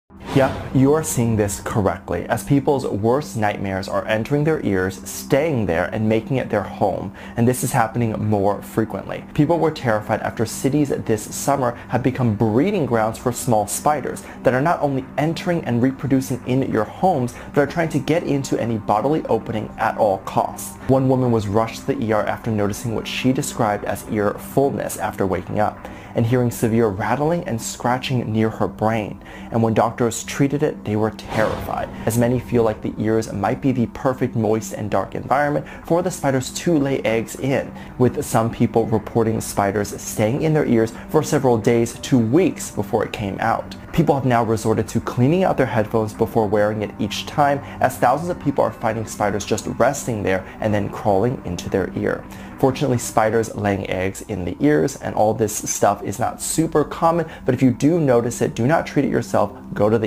Spiders Are Burrowing Themselves in Peoples Ears Now
We explore the rare but real phenomenon of spiders finding their way into human ears. You'll learn about the various species of spiders that have been known to take up temporary residence in the ear canal, and the reasons why they might end up there. We delve into real-life cases, sharing stories from people who have experienced this creepy-crawly situation firsthand. Our experts explain the signs and symptoms to watch out for, and what steps to take if you suspect you have an unwelcome eight-legged guest in your ear. From the initial discomfort to the removal process, we cover it all. You'll also get tips on how to prevent such an occurrence and keep your ears spider-free. Whether you're an arachnophobe or just curious about the strange and unusual, this video is sure to intrigue and educate.